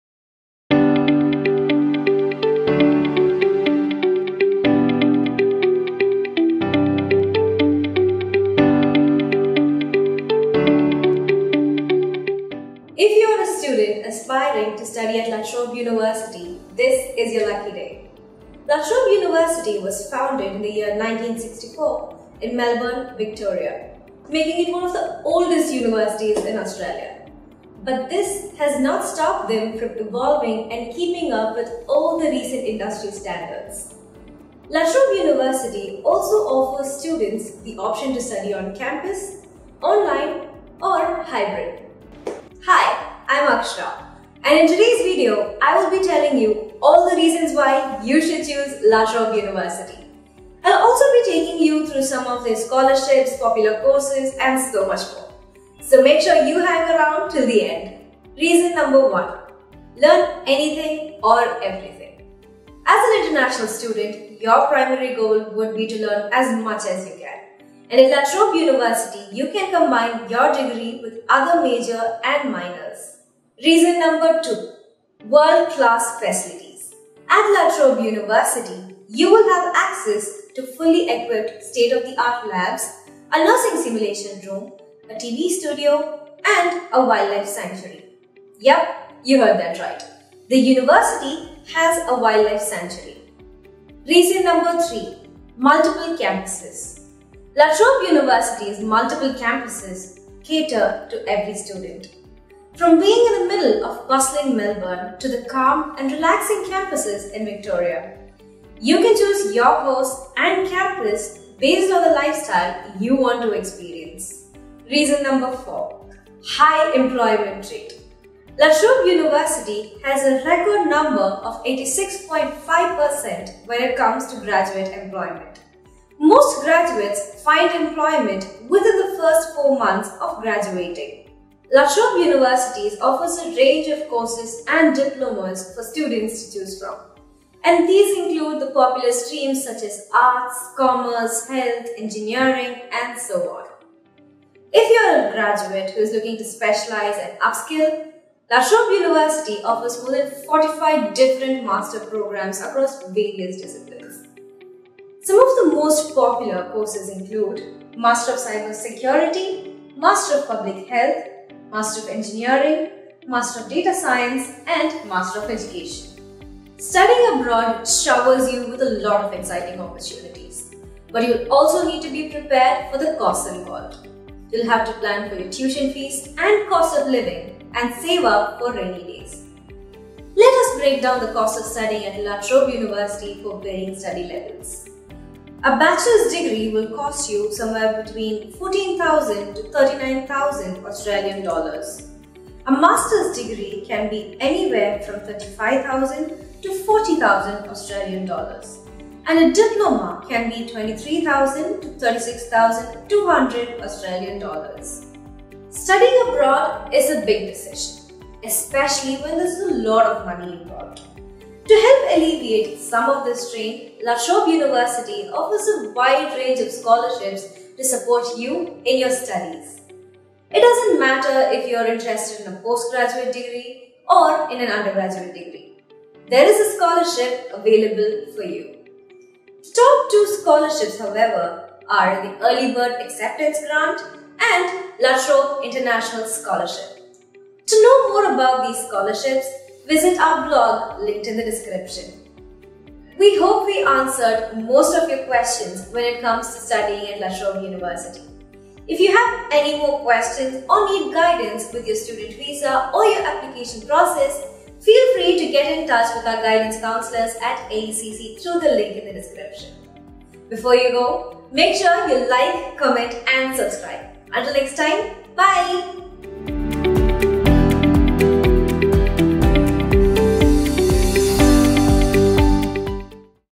Study at La Trobe University | Facilities & Infrastructure | Fees | Courses | Scholarships
In this video you will learn about the various benefits of choosing La Trobe University. Their available courses, fee structure, scholarships and much more.